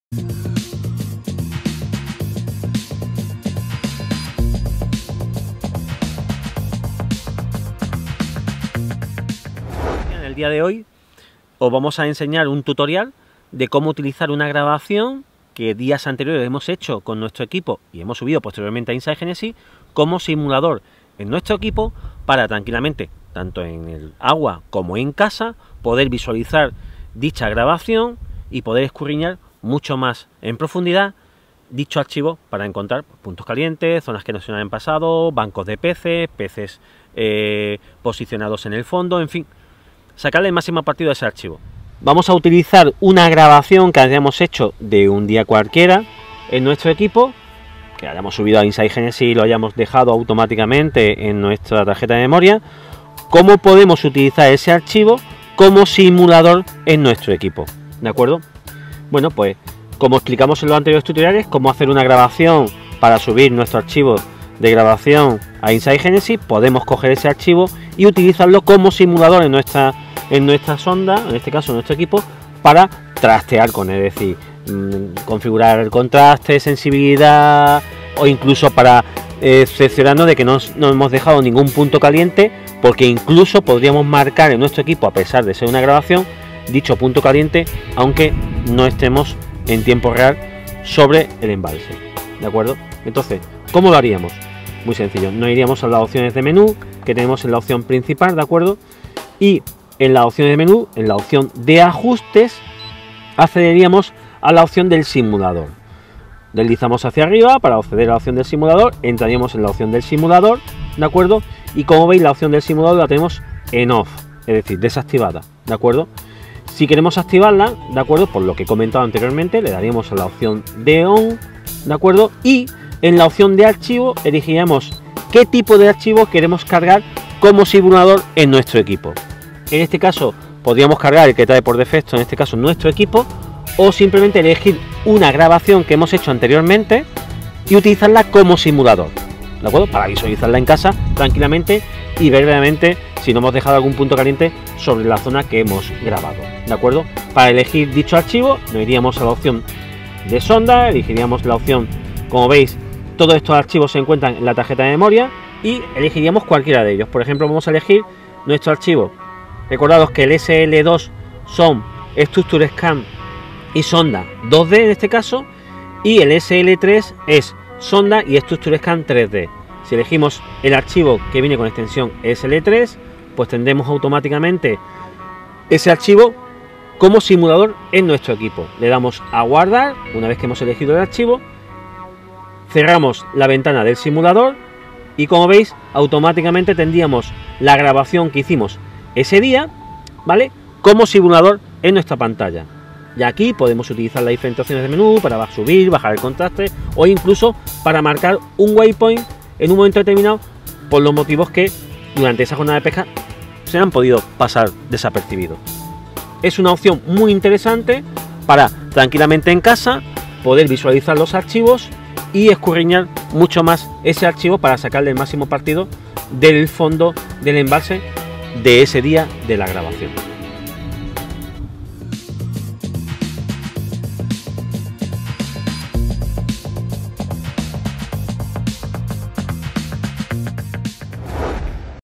ONNautic: Tutorial para la Utilización de una Grabación como Simulador. Lowrance HDS y Elite Ti.
Os presentamos este interesante tutorial como viene siendo habitual desde ONNautic, como utilizar una Grabación como Simulador en nuestros equipos de la gama Elite Ti y HDS.